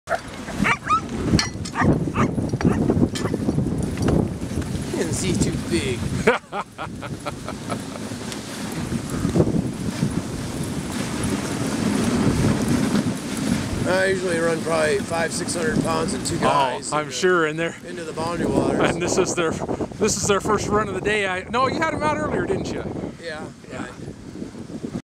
Sled Dog Ride
Silent Run Adventures out of Minnesota giving free rides during Lead Winterfest.